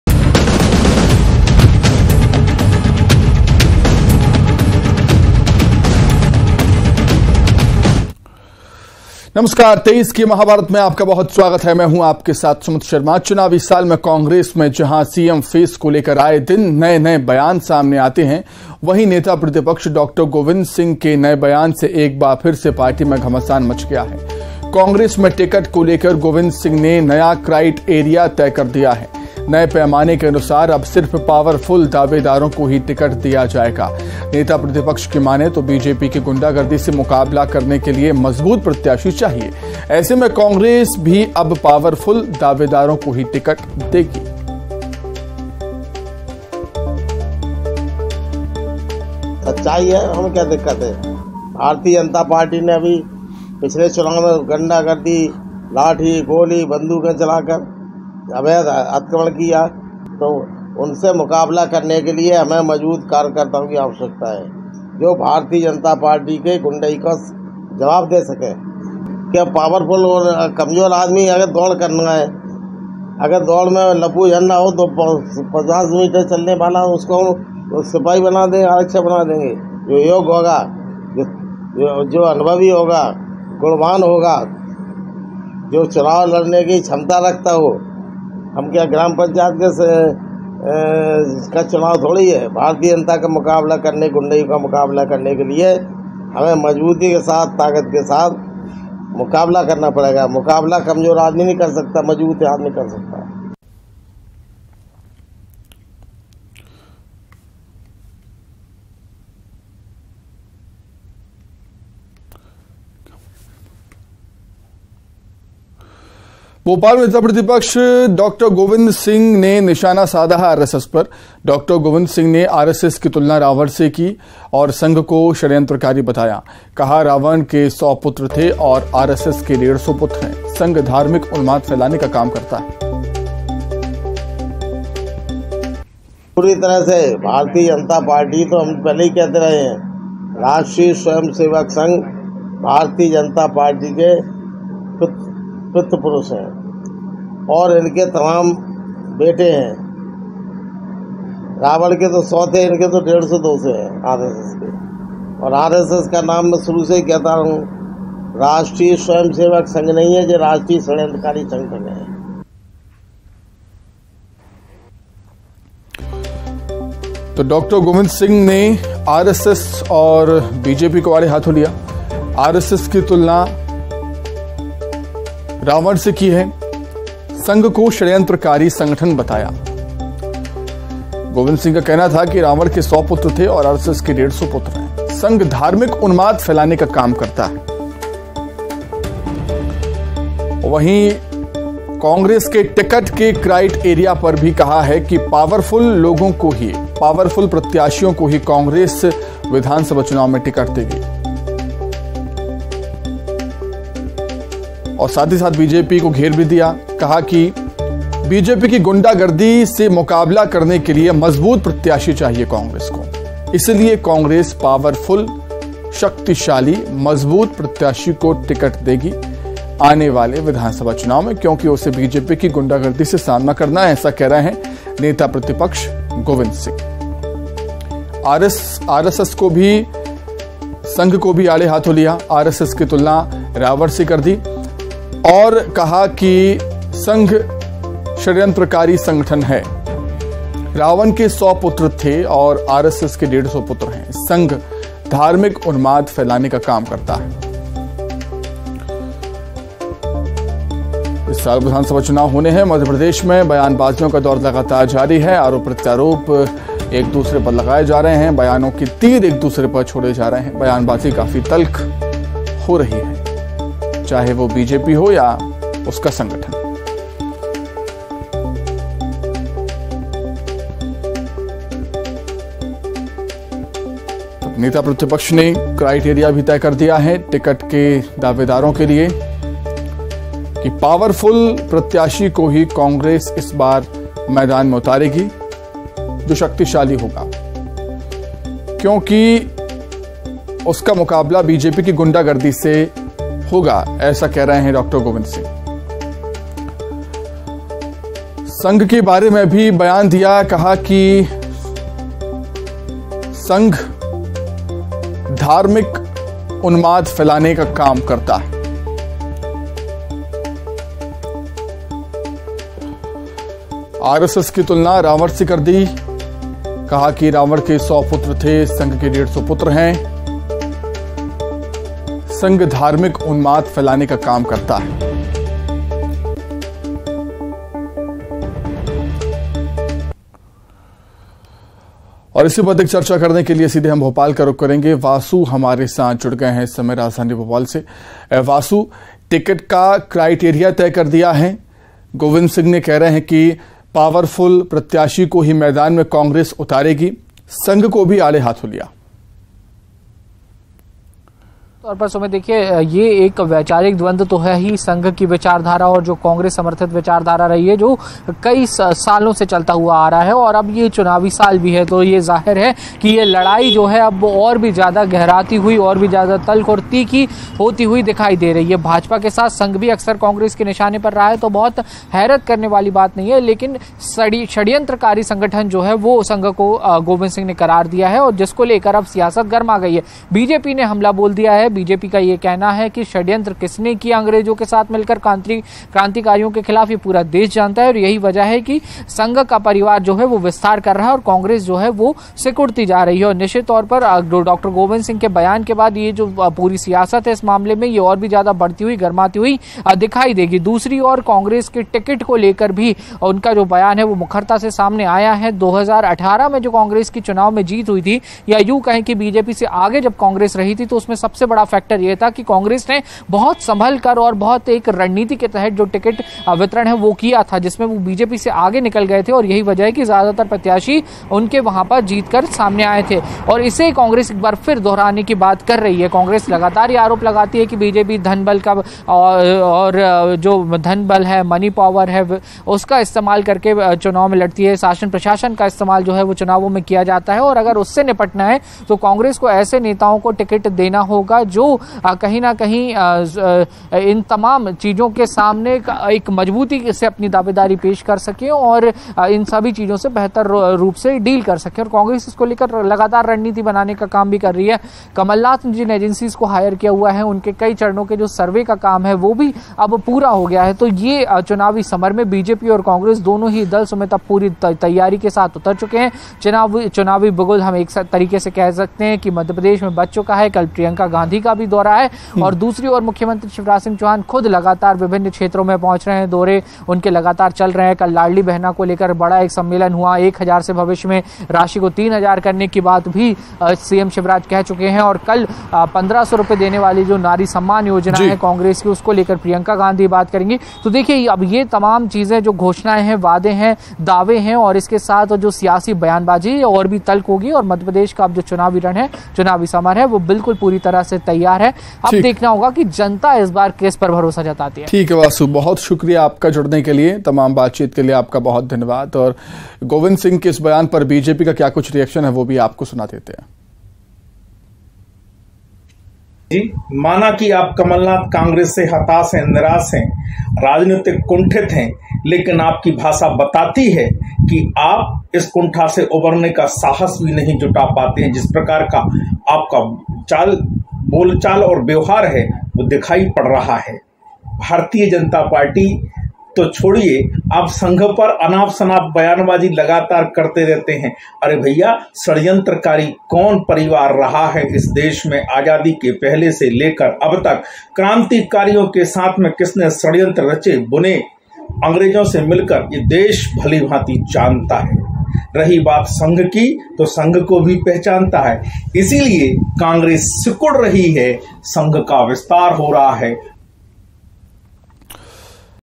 Congress में CM face के लिए आते है नए नए बयान, Dr. Govind Singh के बयान से मचा घमासान | Top News
Copngress में CM face के लिए आते है नए नए बयान, Dr. Govind Singh के बयान से मचा घमासान | Top News